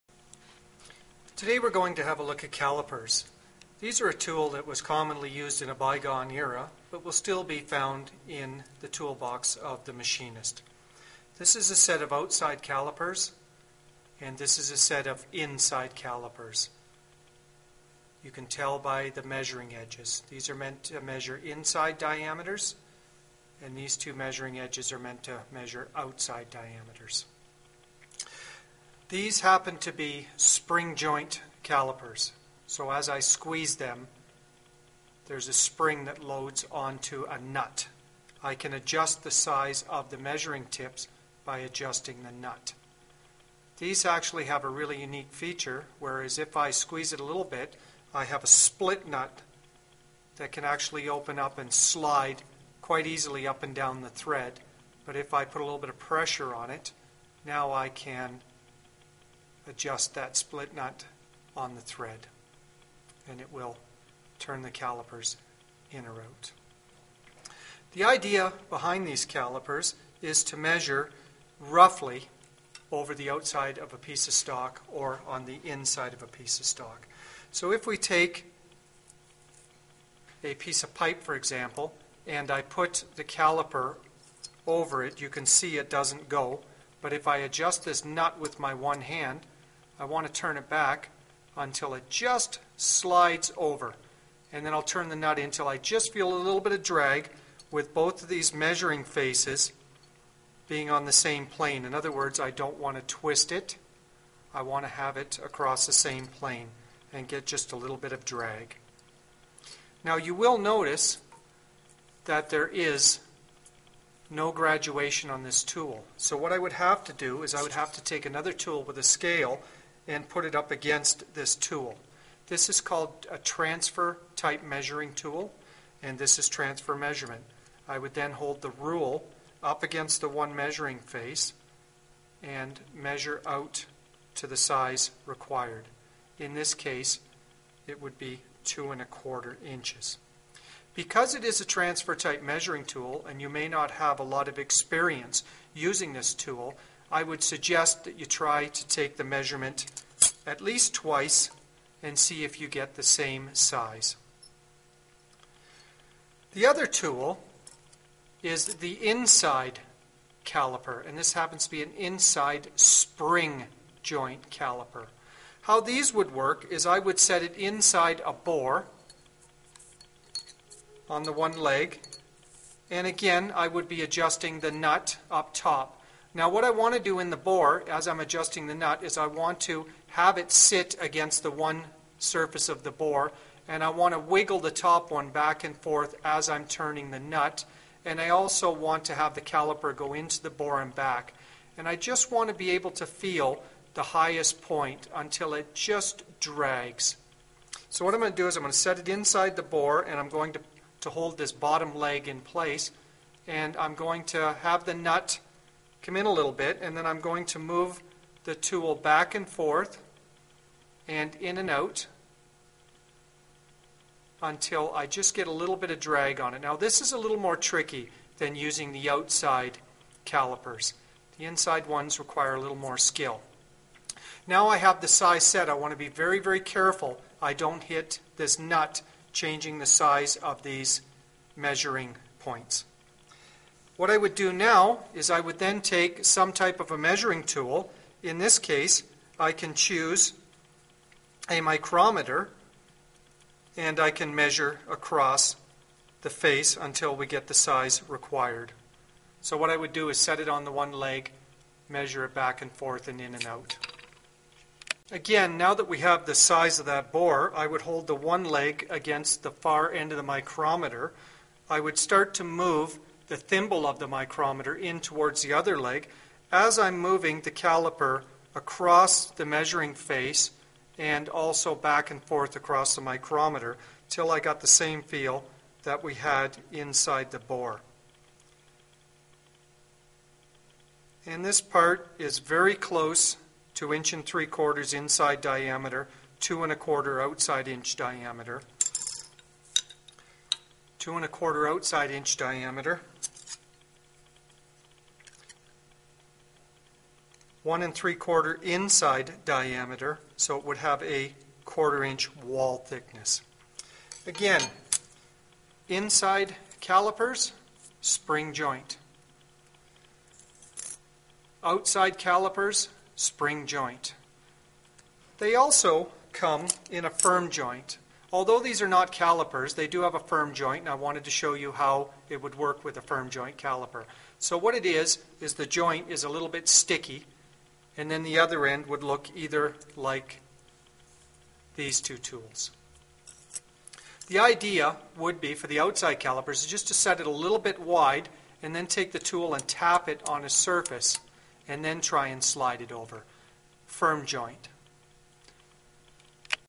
Spring-Joint Calipers inside/outside
How to use calipers to determine part size. Transfering measurements taken.